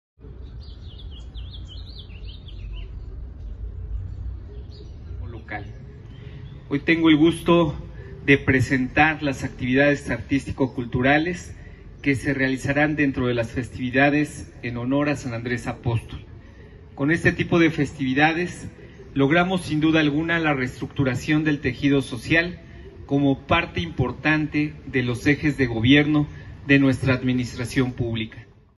Edmundo Tlatehui presenta las actividades artísticas y culturales de la Feria de San Andrés
El alcalde Edmundo Tlatehui presenta las actividades artísticas y culturales de la Feria de San Andrés, que se realizará del 28 de noviembre al 1 de diciembre.